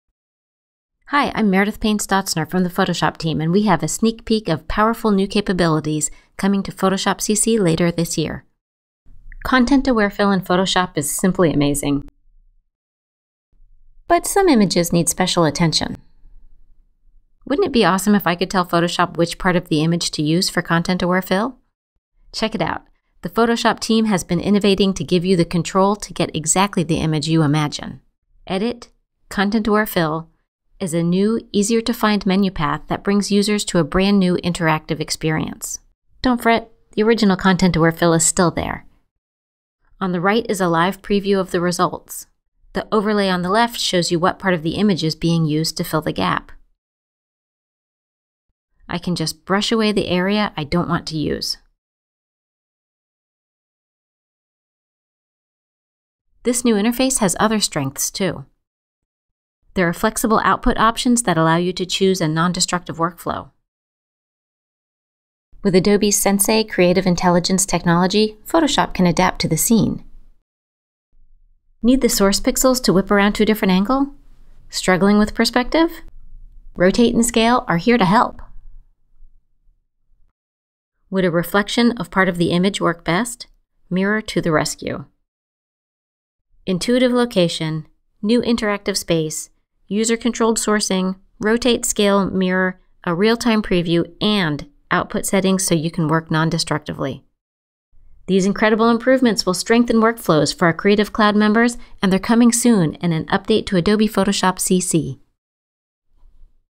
Photoshop Sneak Peek: New Content-Aware Fill | Adobe Photoshop
We’re excited to give you a sneak preview of a powerful, brand-new Content-Aware Fill workspace that will give you more control over your images. Coming soon to Photoshop CC.